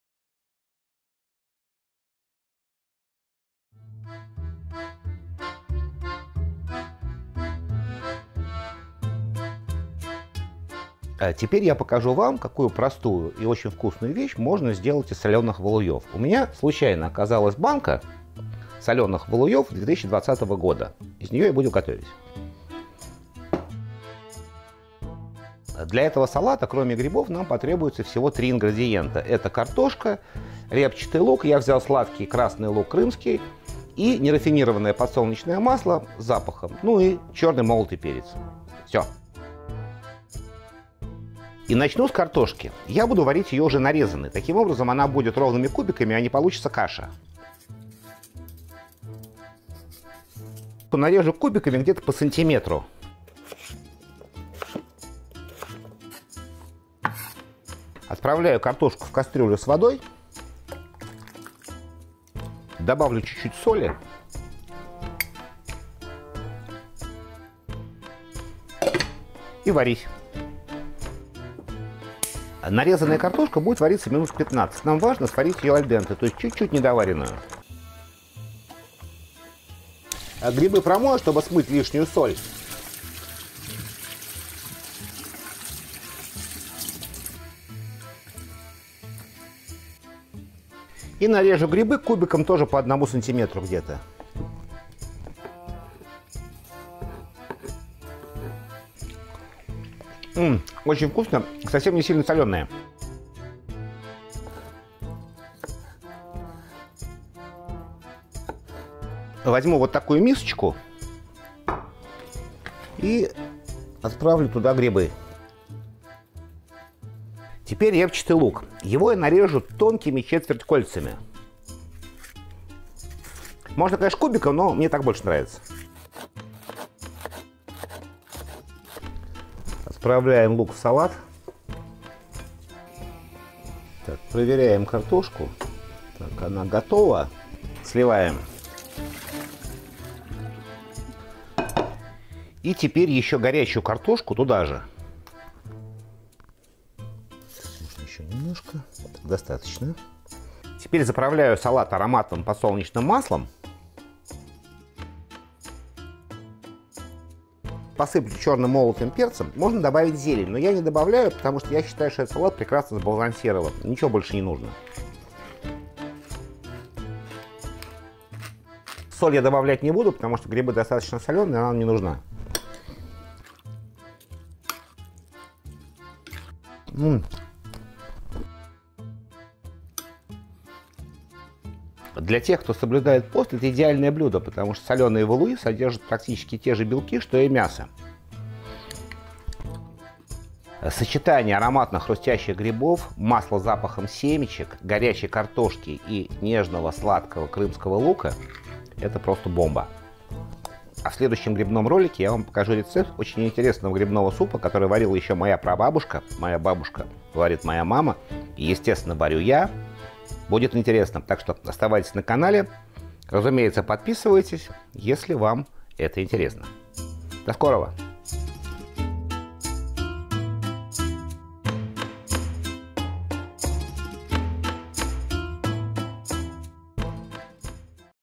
Гриб валуй (бычок). Часть II. Теплый картофельный салат с солеными валуями🥣
Салат с солеными валуями (бычками) с теплой картошкой, сладким хрустящим репчатым луком, заправленный ароматным подсолнечным маслом и черным молотым перцем !  Отличная закуска или полноценное постное блюдо для поста 👩‍🍳

1 пол-литровая банка соленых грибов (у меня валуи)
2 большие картофелины
1-1,5 красных луковиц
60 мл нерафинированного подсолнечного масла